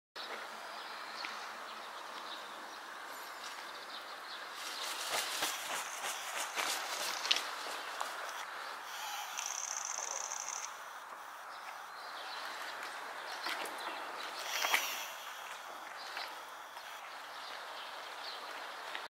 コウノトリ・J0057、和歌山市大谷の用水路から飛び立つ
和歌山市大谷の県営団地脇の用水路護岸から飛び立つコウノトリ・J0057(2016/11/06 AM6:44)